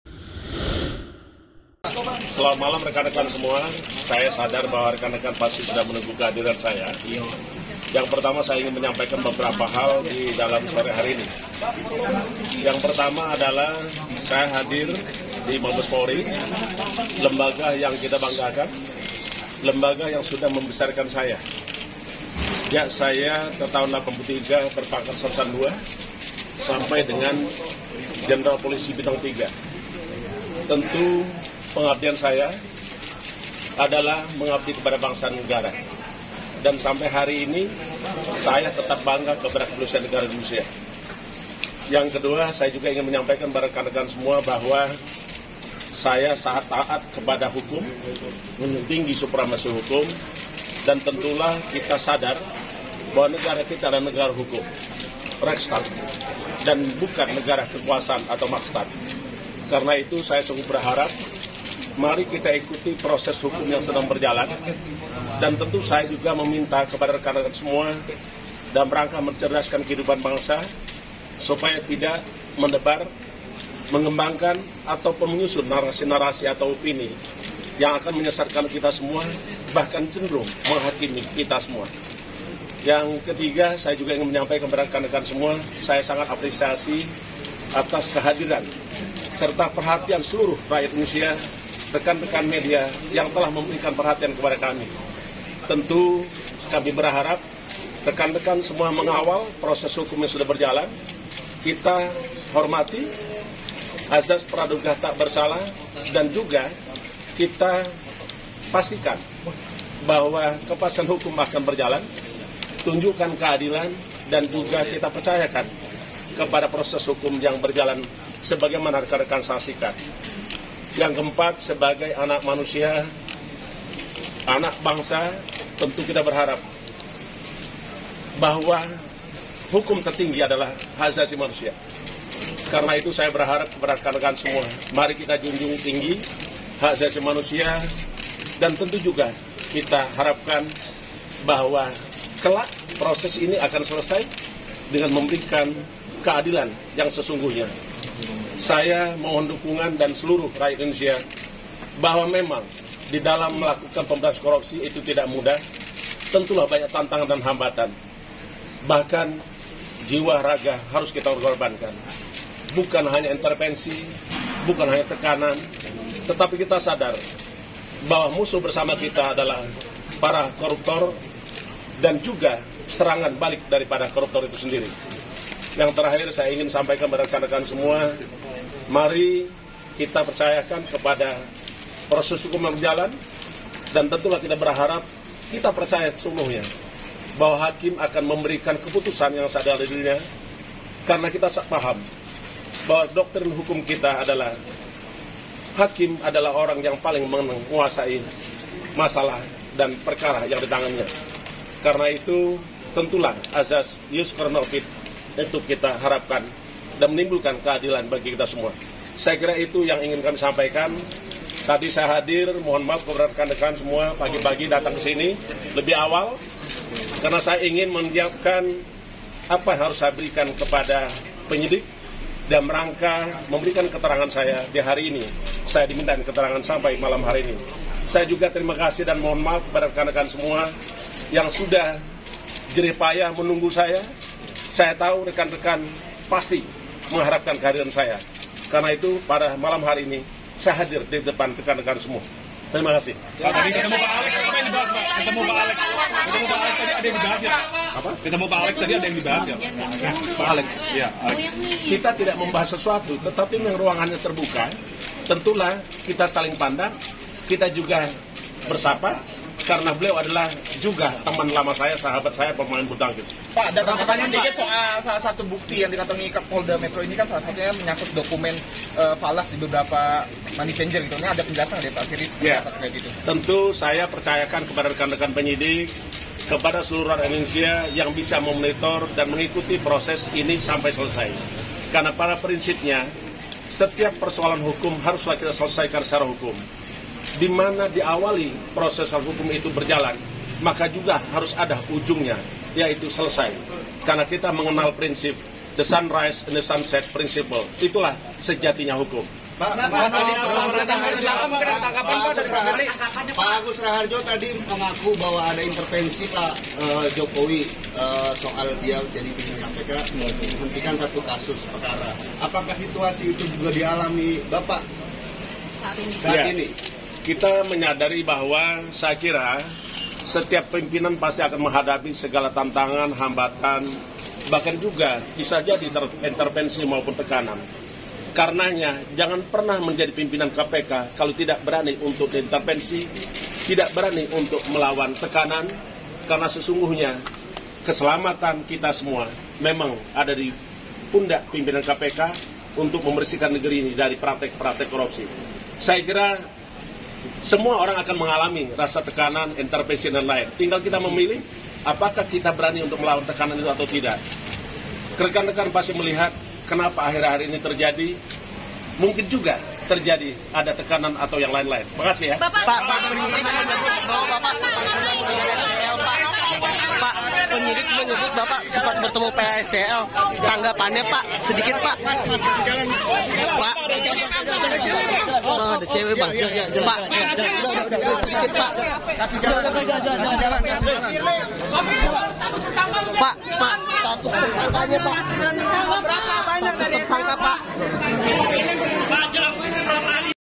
FIRLI BAHURI Ditetapkan Sebagai Tersangka Kasus Dugaan Pemerasan, Kini Di Sindir Oleh Para Koruptor
Tersangka kasus dugaan pemerasan Firli Bahuri menyindir soal status dirinya sebagai Ketua KPK yang banyak mendapat tekanan dan intervensi dari berbagai pihak.

Termasuk penetapan tersangka kepada dirinya dalam perkara dugaan pemerasan terhadap mantan Mentan, Syahrul Yasin Limpo.

Kepada awak media Firli juga meminta dukungan kepada publik dalam perkara pemerasan yang menimpa dirinya.

"Bahwa memang di dalam melakukan pemberantasan korupsi itu tidak mudah, tentulah banyak tantangan dan hambatan, bahkan jiwa raga harus kita korbankan," katanya kepada wartawan, Jumat (1/12/2023).

Laporan Pandi